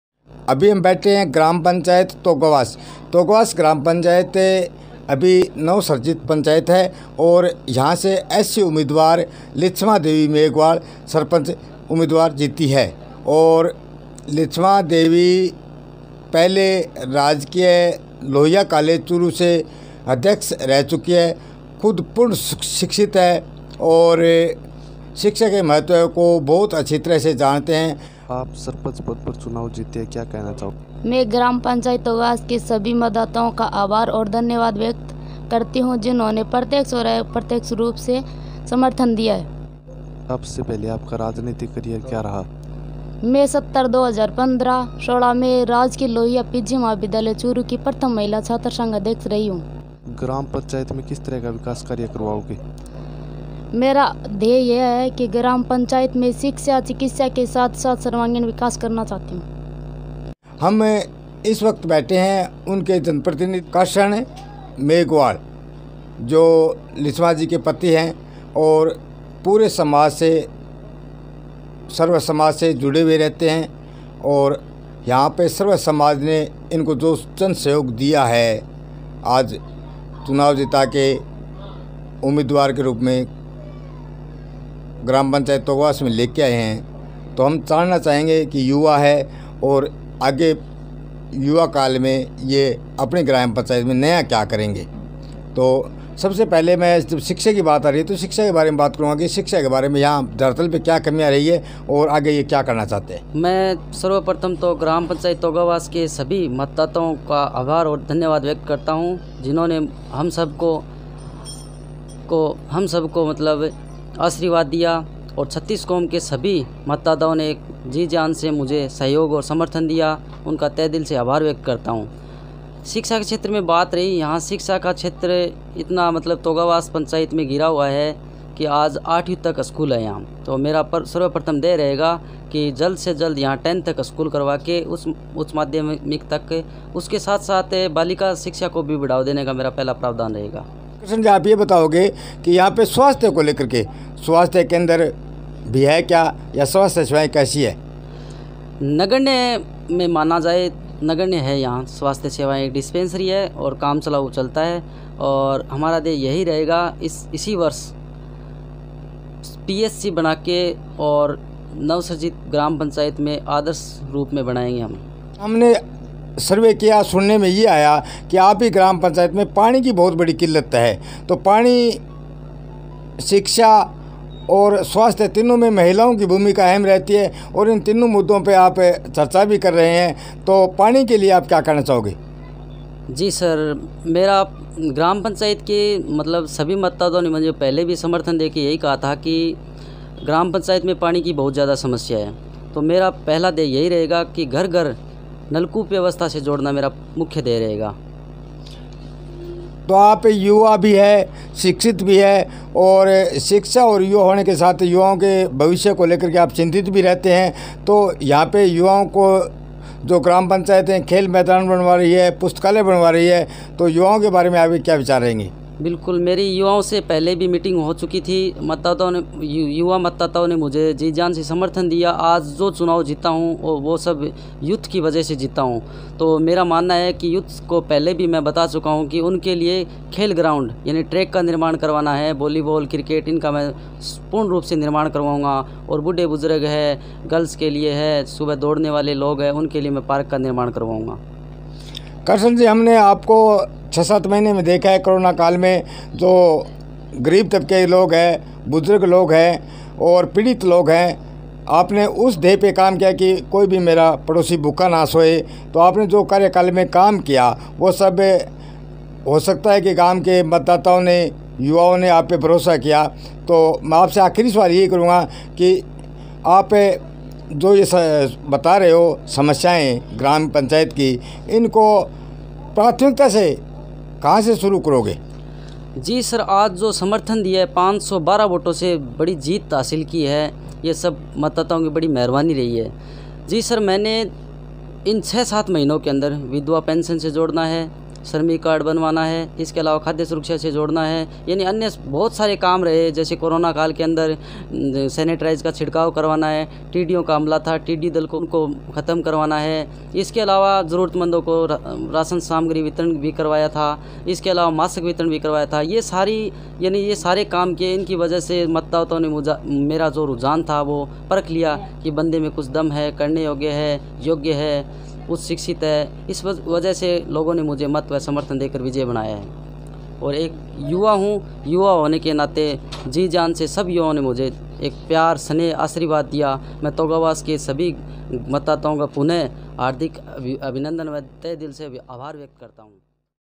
तारानगर। तोगावास से सरपंच बनी पूर्व कॉलेज अध्यक्ष लिछमा मेघवाल,कही ये बड़ी बातें,बहाएंगे विकास की..!
सरपंच बनने के बाद लिछमा मेघवाल व जनप्रतिनिधि कृष्ण मेघवाल की जागो हुक्मरान न्यूज़ से खास बातचीत।

तोगावास से सरपंच बनी पूर्व कॉलेज अध्यक्ष लिछमा मेघवाल,कही ये बड़ी बातें,बहाएंगे विकास की गंगा..!